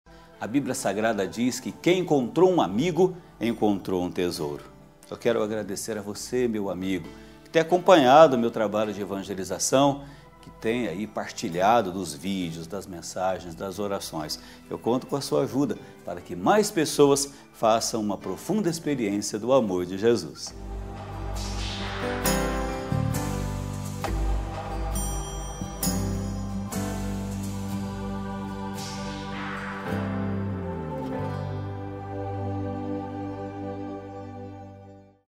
OBRIGADO aos amigos das redes sociais
Diariamente vocês têm sido sinal e presença de Deus no mundo virtual.

Obrigado por evangelizarem comigo todos os dias!